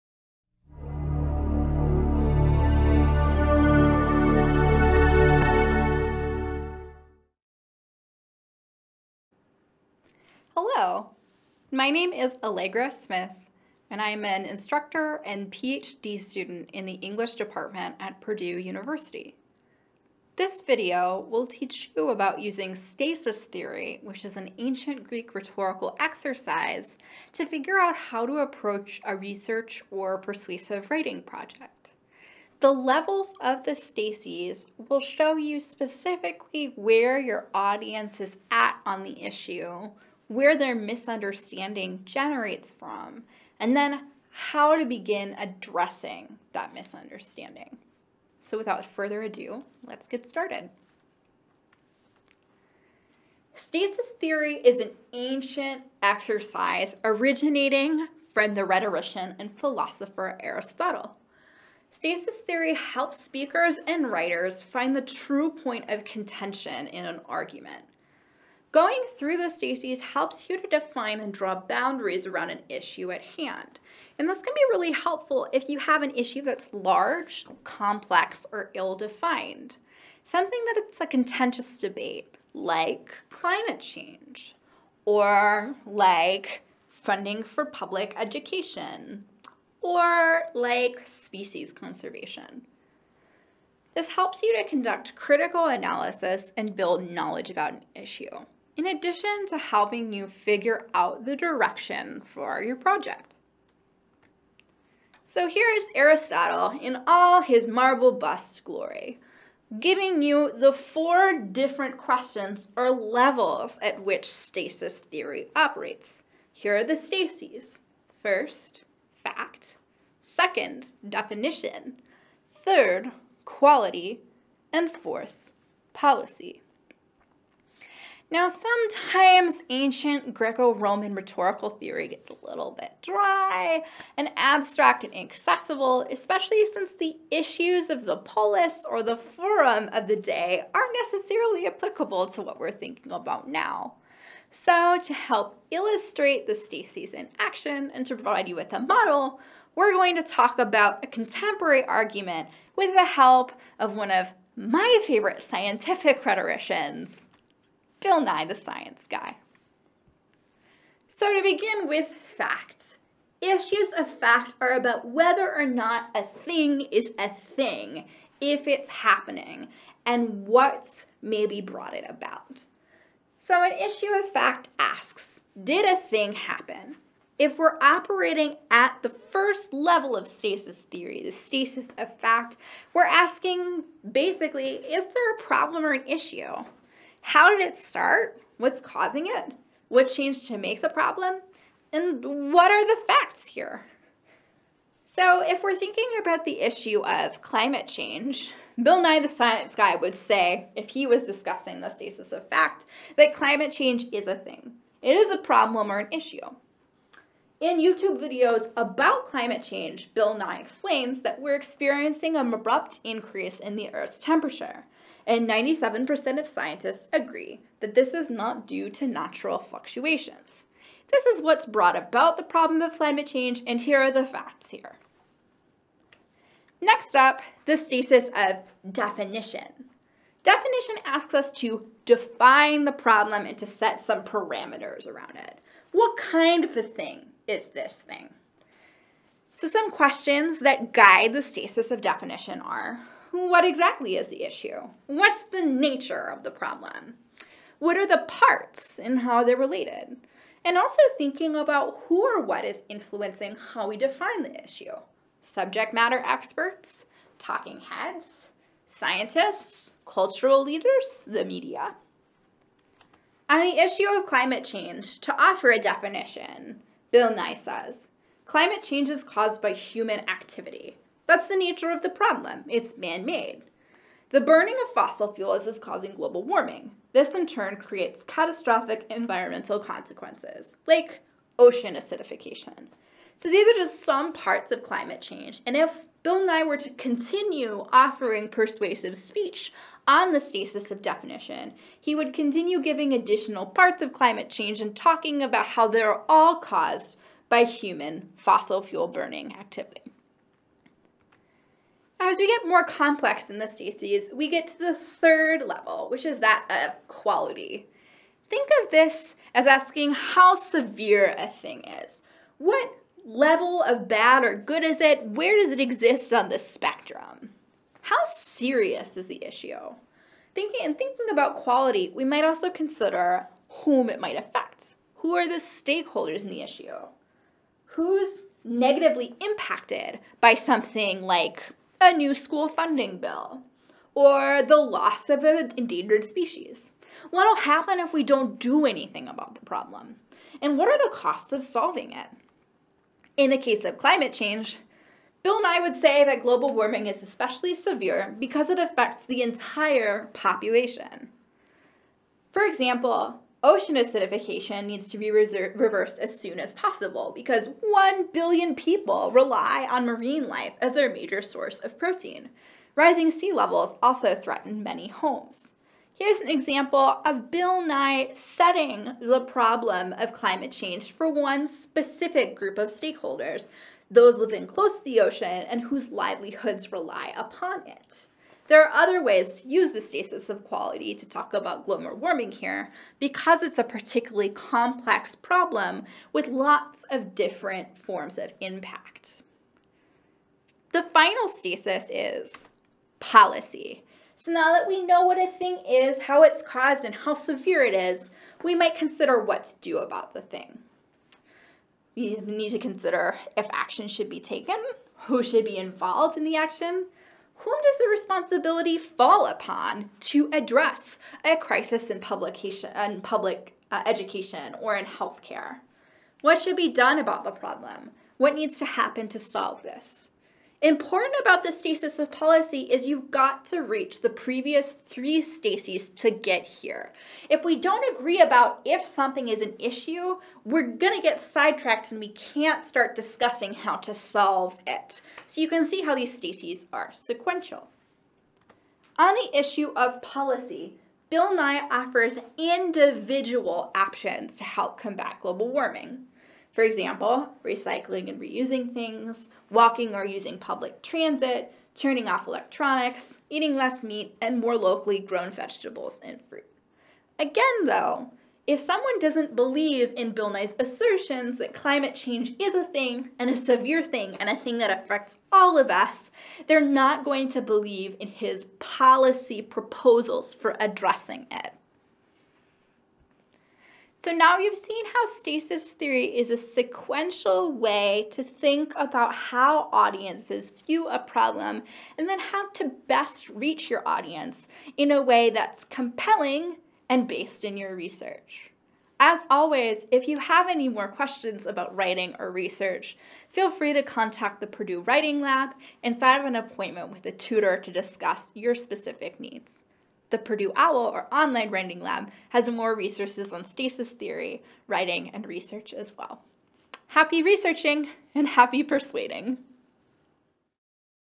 How Do I Approach a Research Project for My Writing Class? Stasis Theory for Narrowing Topics
Have an idea for a research topic, but don't know how to approach it? Unsure of the best way to reach your target audience? The classical rhetorical framework of stasis theory can help you to better understand the topic that you're working with, and how to target your argument to your audience. This video explains and offers a model for applying the four stases of 1) fact, 2) definition, 3) quality, and 4) policy. (Allegra W. Smith for Professional Writing at Purdue University)